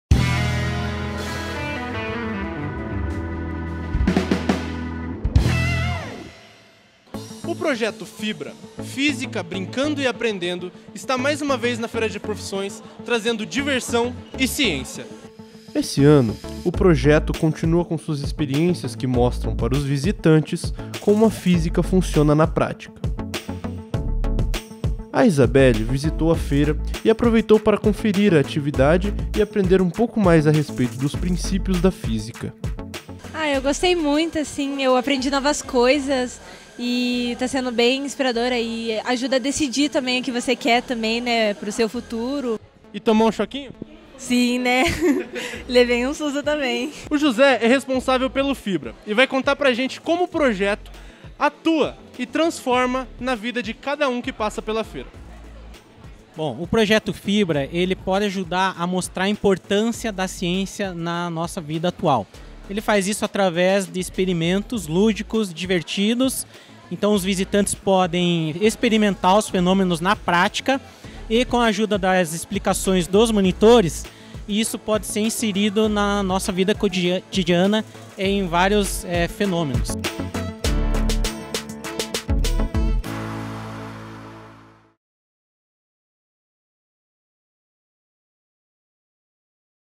UFPR: Cursos e Profissões | Física Brincando e Aprendendo
Diversão e ciência andam juntas com o projeto FiBrA - Física Brincando e Aprendendo! A ideia é mostrar aos visitantes da Feira de Cursos como a física pode ser compreendida na prática, de uma forma lúdica e bem diferente. Para conhecer mais sobre o projeto e seus experimentos, conheça o programa FiBrA da UFPR TV

💡 A UFPR: Cursos e Profissões acontece entre os dias 18 e 21 de agosto de 2022, no Complexo UFPR Piraquara - Rod. Dep. João Leopoldo Jacomel, 4015. Para mais informações sobre o evento, acesse